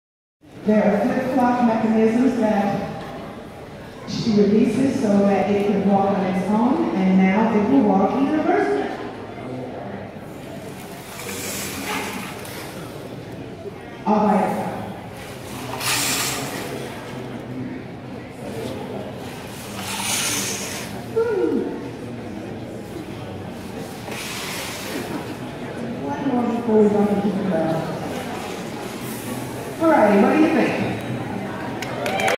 Strandbeest at the Exploratorium
Theo Jansen's Strandbeest exhibit is currently on display at San Francisco's Exploratorium.